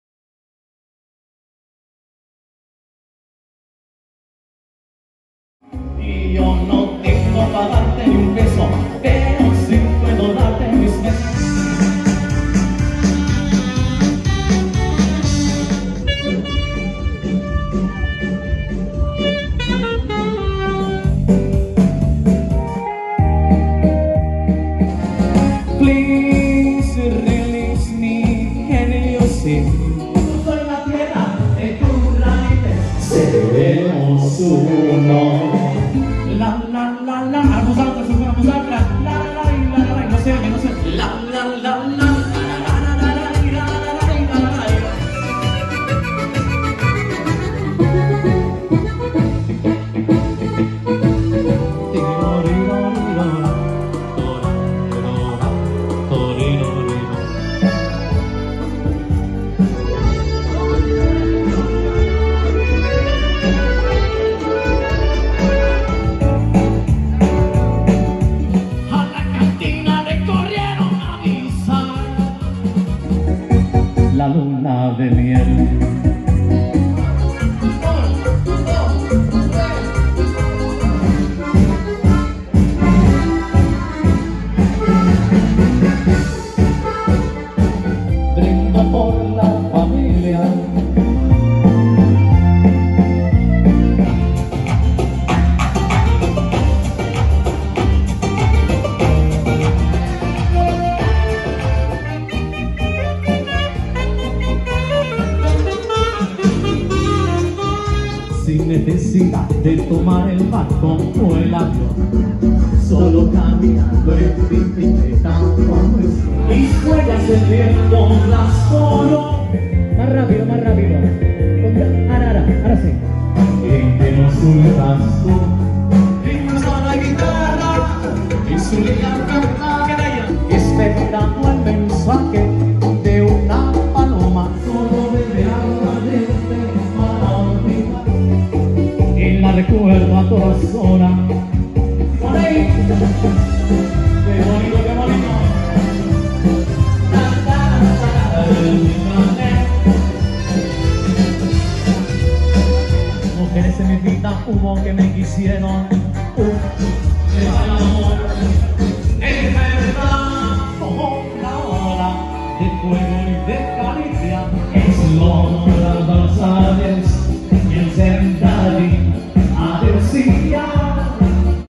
Ball de tarda a Solsona
Ball de tarda al centre cívic de Solsona el 24/04/22. Moltes gràcies!